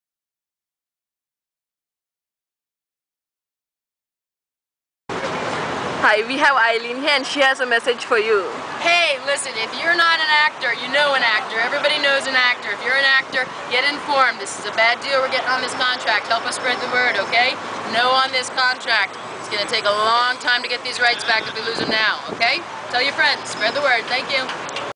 Eileen's Message
SAG member Eileen has a message for people and SAG members about the Contract